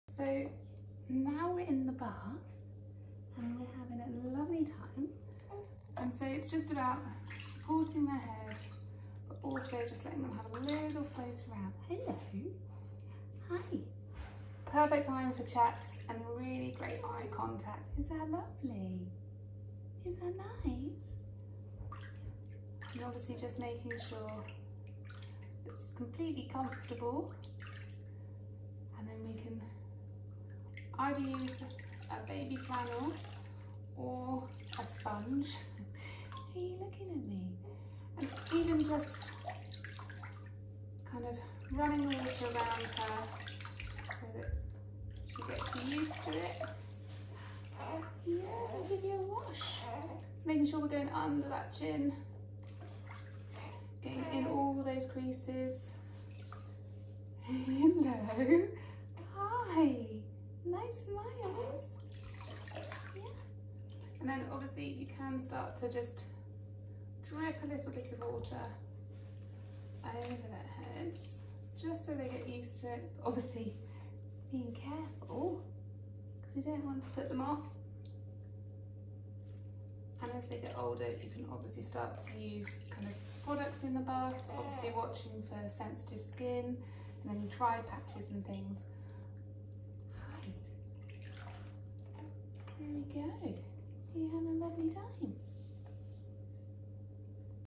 Bathing baby
Some hints and tips from Claire for bathing your baby.